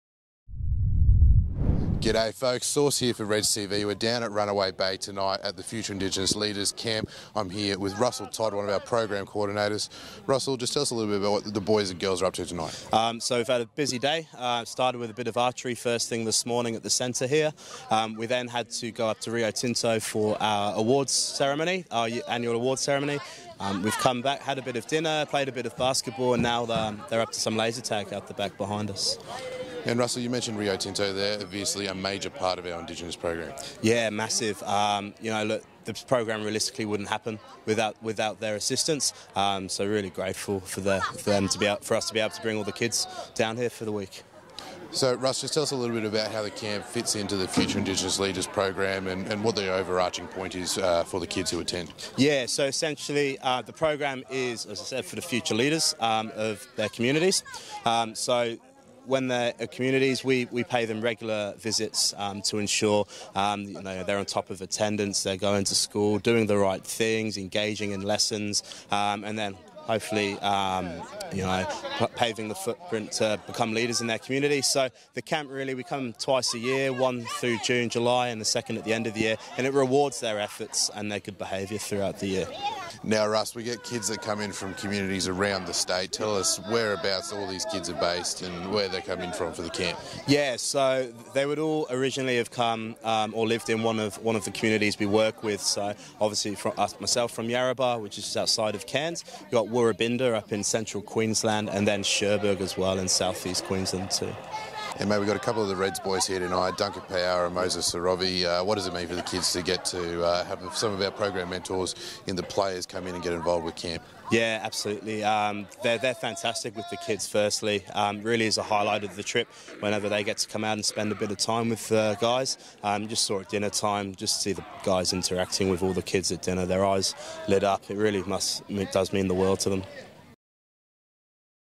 Future Indigenous Leaders Camp 2018
This week our Future Indigenous Leaders program participants have converged on Runaway Bay at the Gold Coast for their second annual camp of 2018.

The kids have had a busy week, taking part in some archery, visiting Wet 'n' Wild, getting involved in some Surf Lifesaving activities, trying their hand at Laser Tag, and visiting our major program partners Rio Tinto Australia.

Reds TV joined the kids in camp this week alongside program ambassadors Duncan Paia'aua and Moses Sorovi.